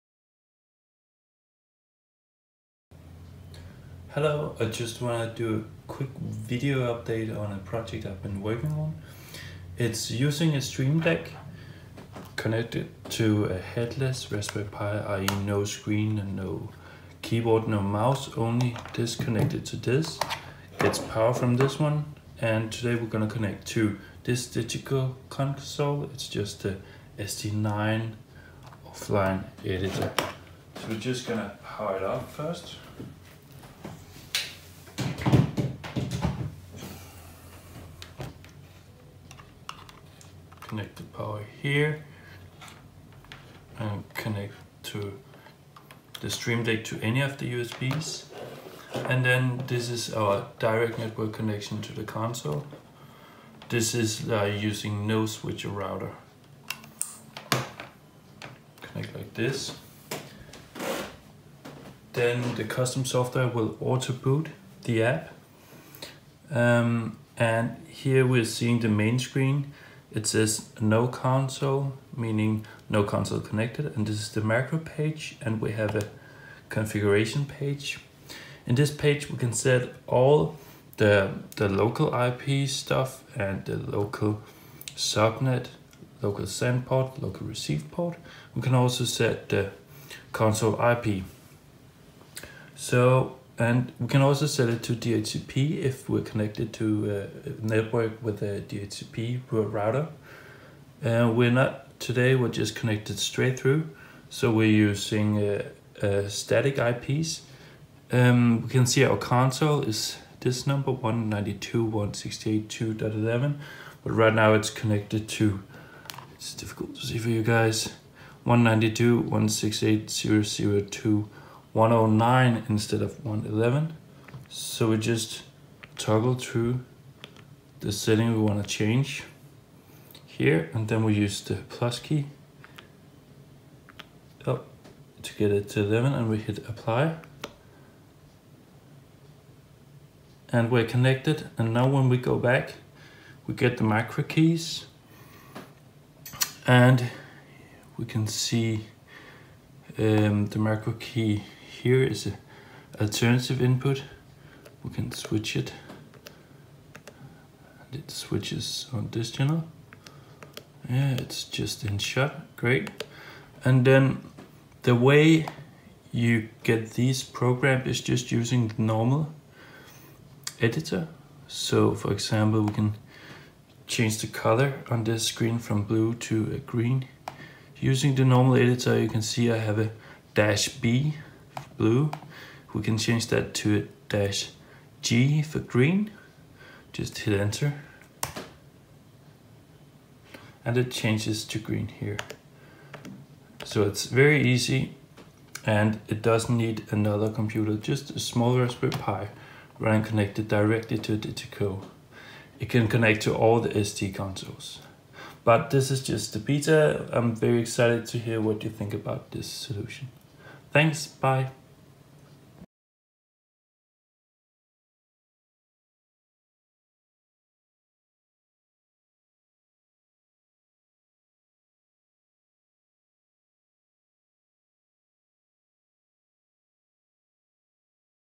#5 DiGiDeck Beta presentation
This video shows a presentation of controlling the macros on DiGiCo SD digital audio live consoles using a Stream Deck and a Raspberry Pi with DiGiDeck beta software by Frostbox Labs.

This is the easiest solution to get nice colour lcd macros on DiGiCo consoles and can be placed anywhere on the stage connected through normal cat5e cabling.

The software is developed to make it as easy to use as possible. When it is powered it autoboots to the last used settings and all network configuration options are easily accessable using the Stream Deck controller - no need for external laptops etc. It uses the DiGiCO Ipad OSC protocol and integrates with the normal macro editor in the DiGiCO SD software and provides the option to add background colours based on the macro name using "/g" for green as an example.

labs.frostbox.net